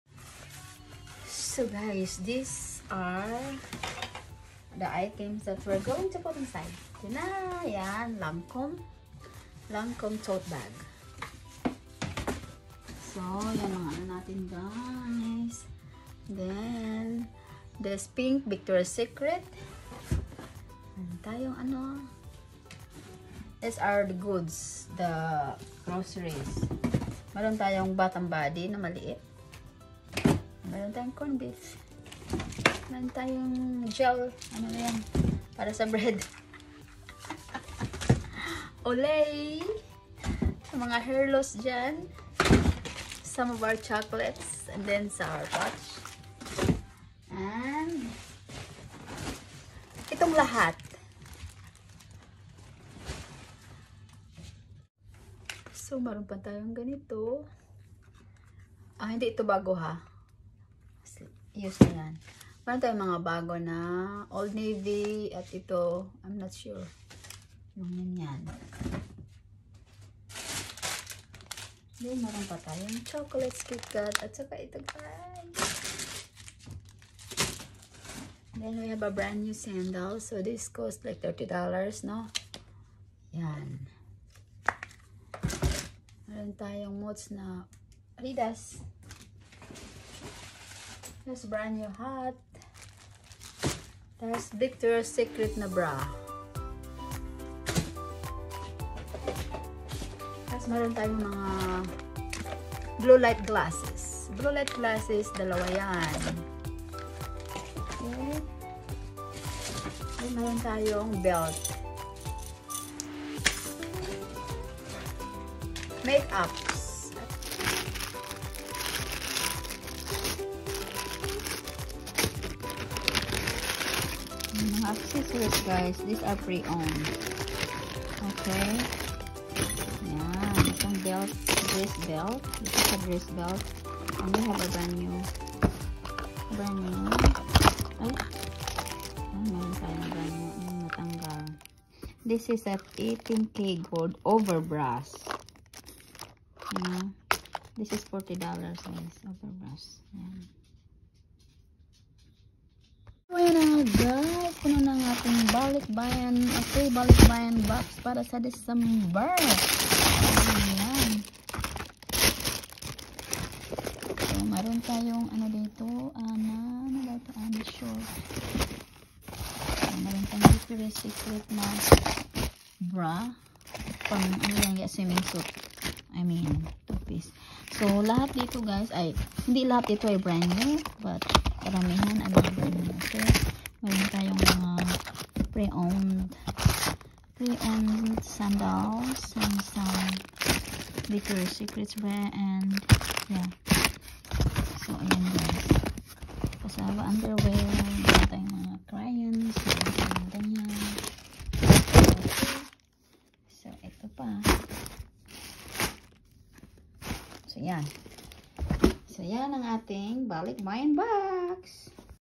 What’s INSIDE to our FREE SMALL BALIKBAYAN BOX GIVE AWAYS in DECEMBER
On december THE BELDENS will have FREE SMALL BALIKBAYAN BOX GIVEAWAYS to  all LEGIT SUPPORTERS of our  Different PLatform  PAGES. like THE BELDENS PAGE on FB , and to our THE BELDENS Yt channel. Paano ba mkasali? INVALID ang panalo pag di  nka follow
2.)Dapat lagi ko makikita sa comments  section ng ating mga reels at  video sa YT  channel  para di makalimutan  malista sa ating Roleta sa DECEMBRE!